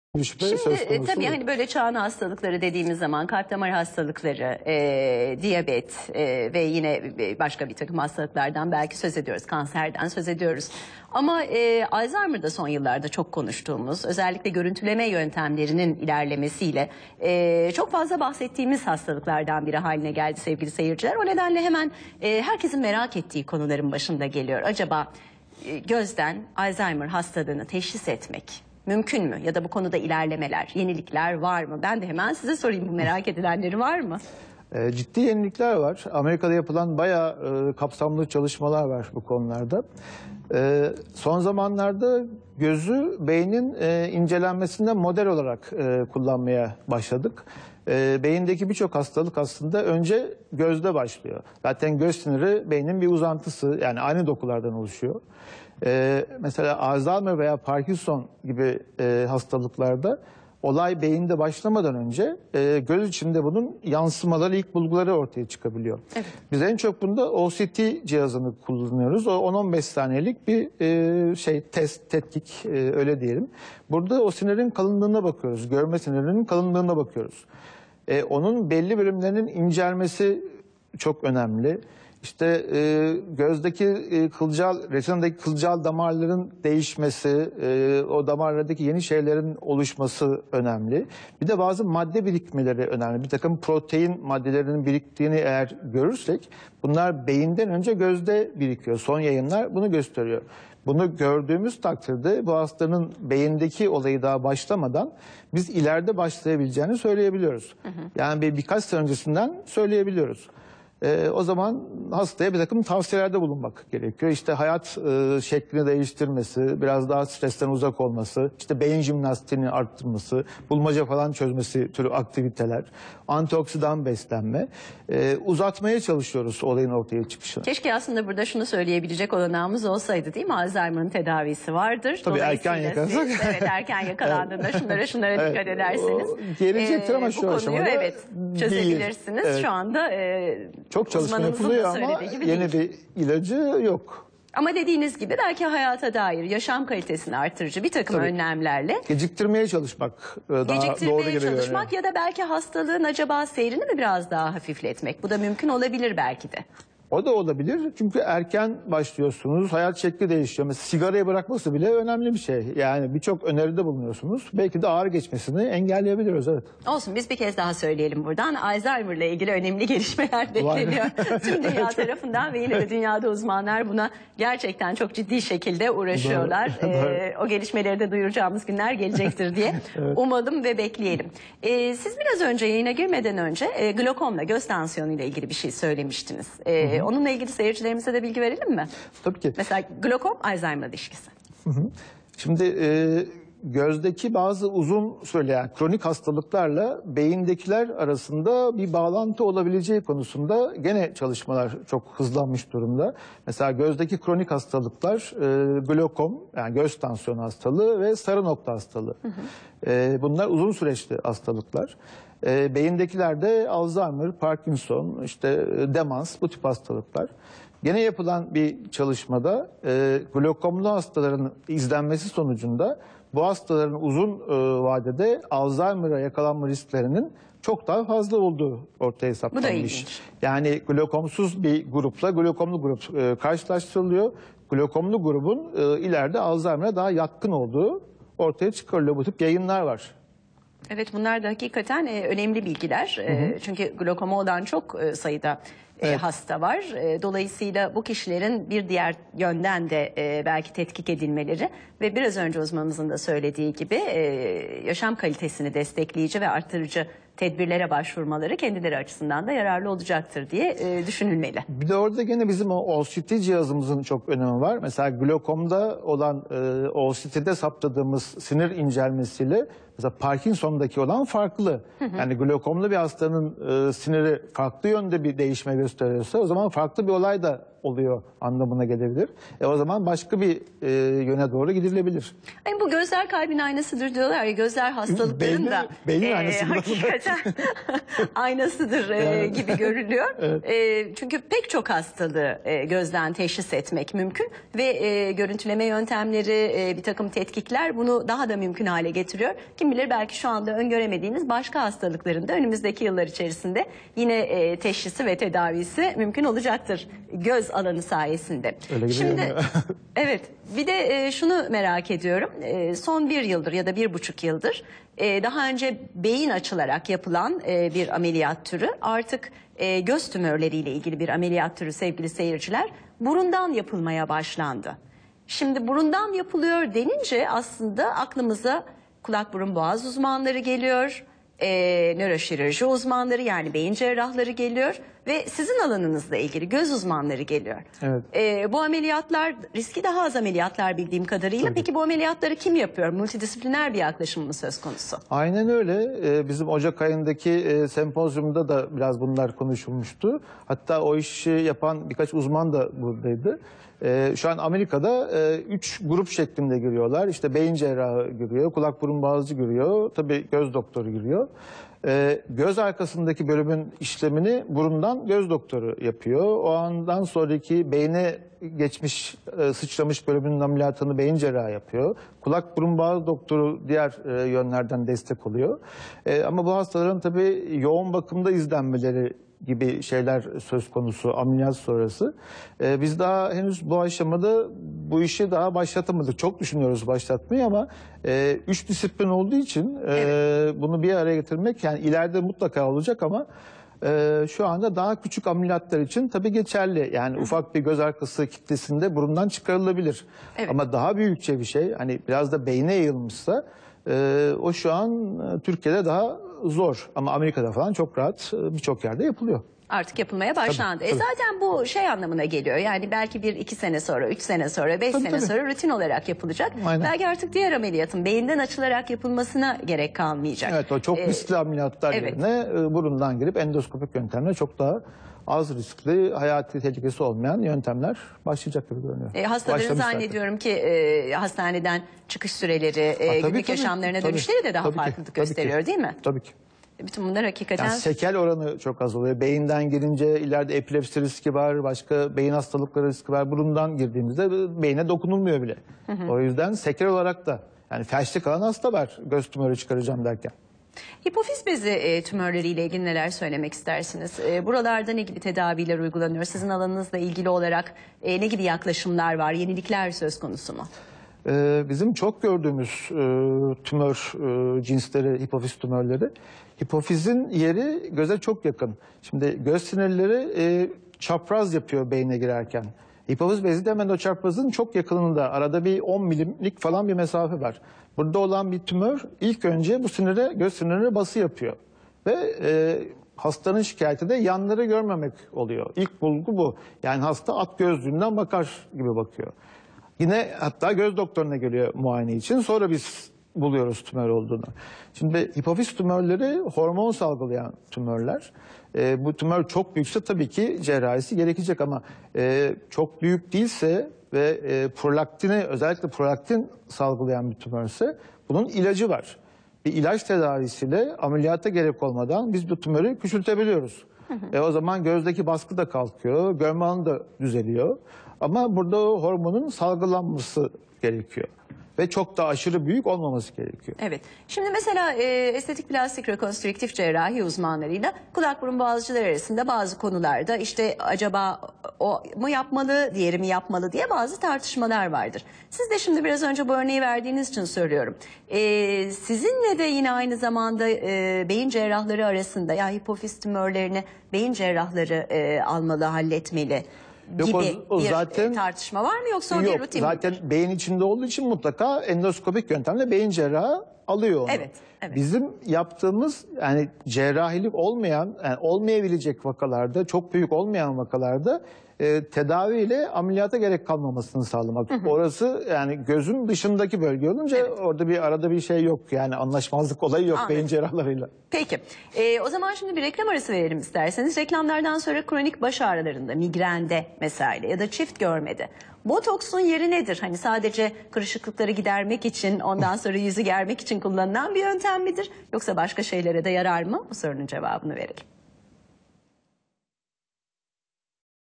Ms hastalarında ilk belirti gözde mi ortaya çıkıyor ?
Ceyda Erenoğlu'nun hazırlayıp sunduğu HT Sağlık'a konuk olan Opr. Dr. Umur Kayabaşı, göz siniri sorunlarını anlattı.